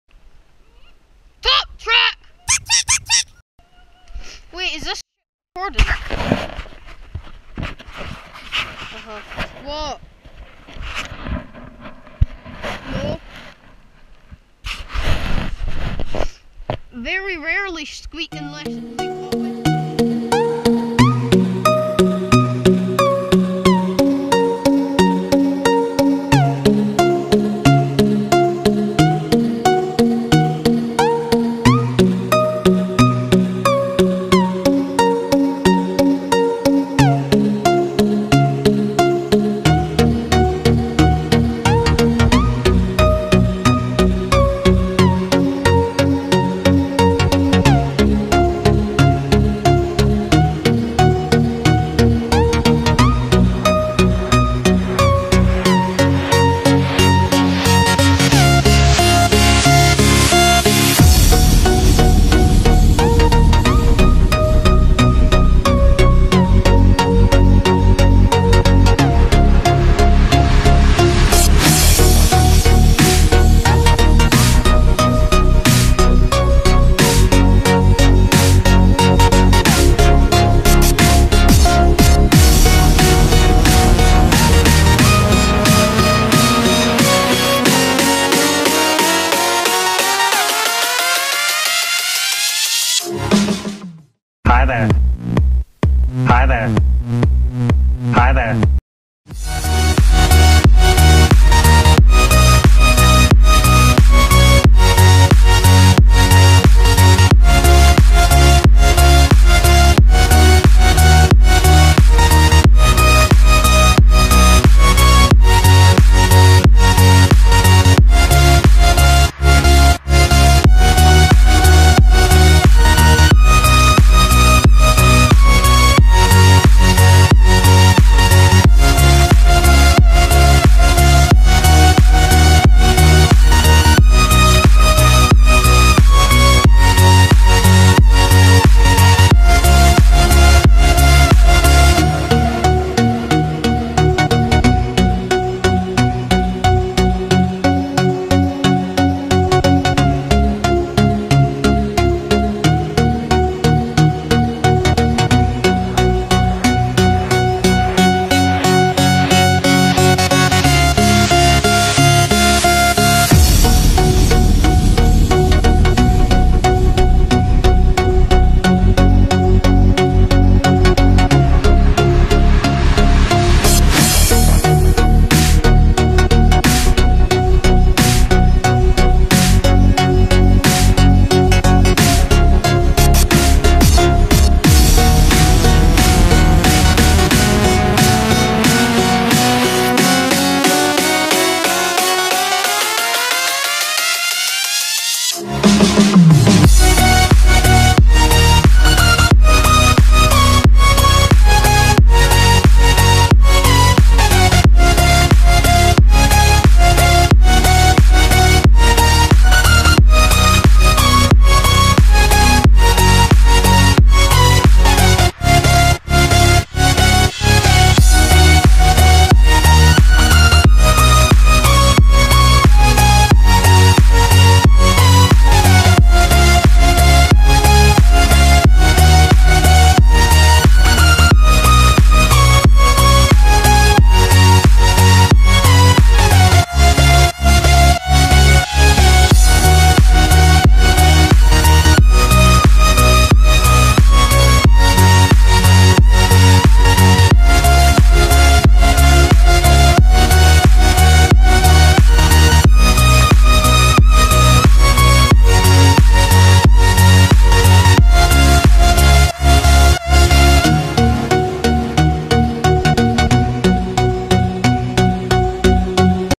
Kinneil top track
Us ( max and keir) go to kenneil woods top track with Ben and Kieran